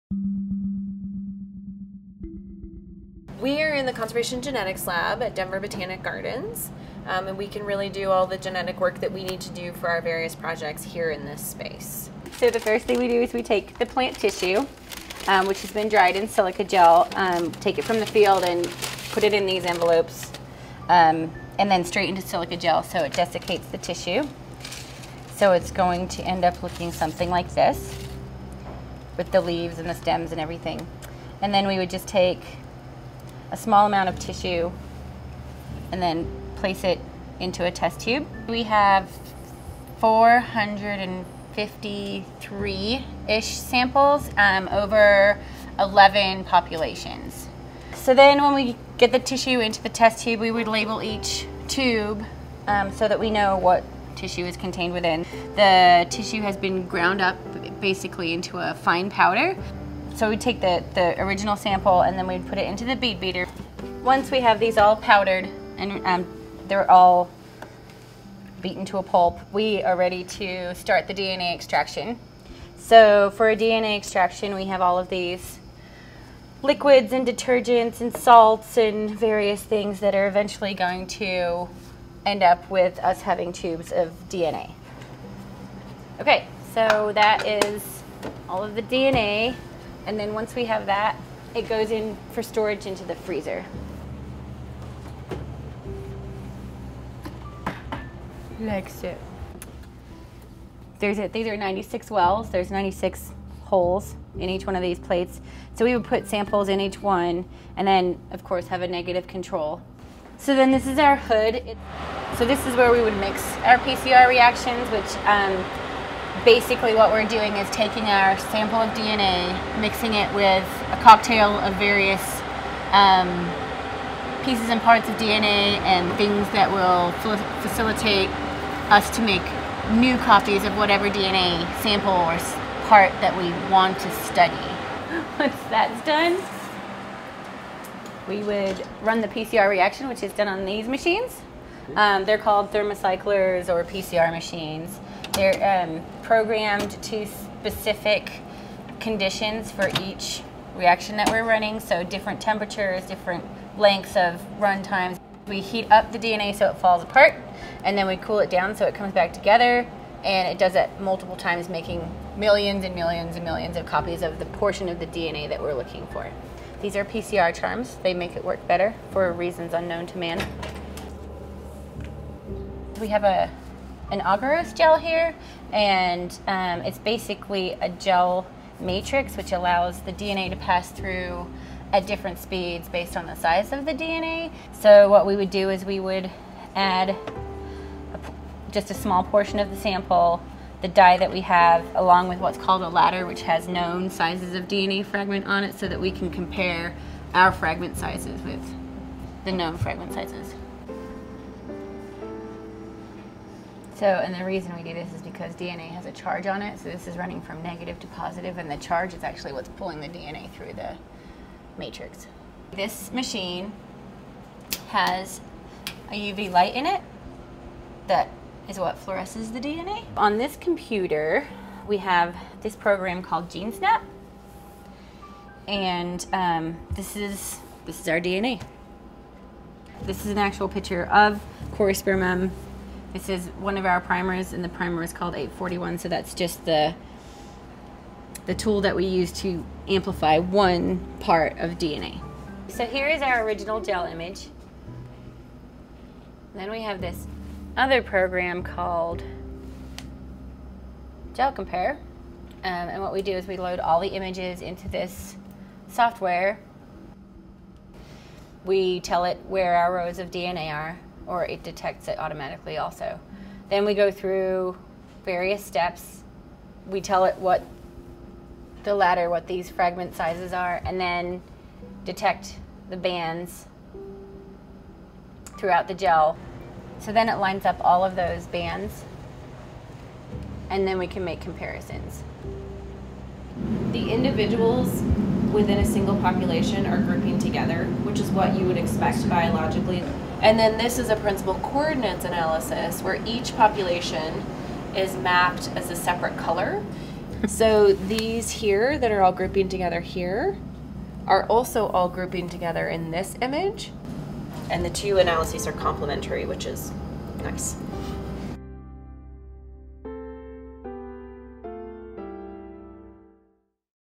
The Lab at Denver Botanic Gardens
Walk step by step through the process of making genetic comparisons between different plants. Denver Botanic Gardens' genetics lab technician, Anna Schwabe, extracts genes and demonstrates the methods required to achieve meaningful data.